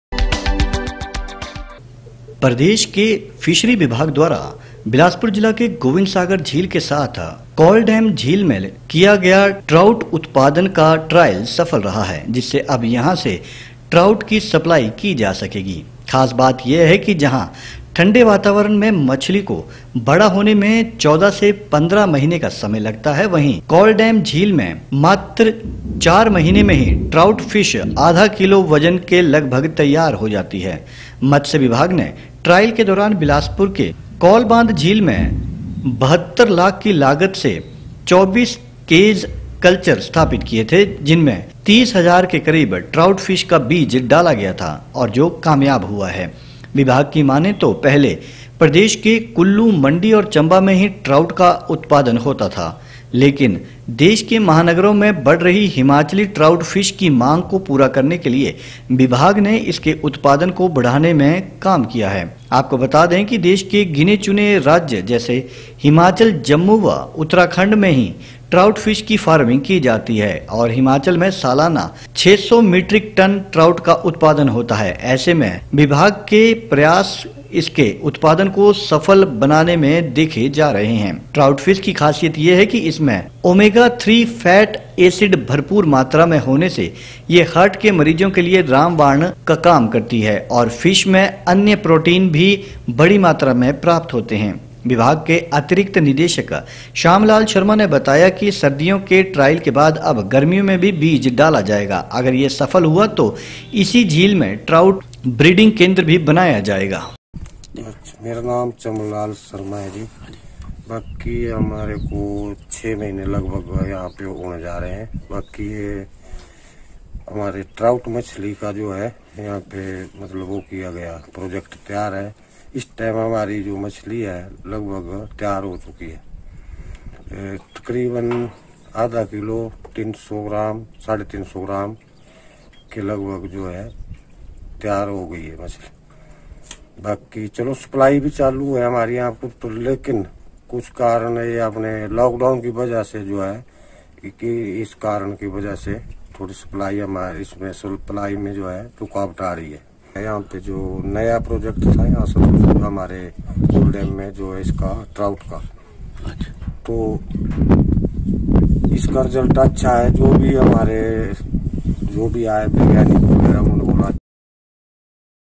28 April फिशरी विभाग द्वारा बिलासपुर जिला के गोविंदसागर में किया गया ट्राउट उत्पादन का ट्रायल सफल
प्रदेश के फिशरी विभाग द्वारा बिलासपुर जिला के गोविंदसागर झील के साथ साथ कोलडैम झील में किया गया ट्राउट उत्पादन का ट्रायल सफल हो गया है। जिससे अब यहां से भी ट्राउट की सप्लाई की जा सकेगी।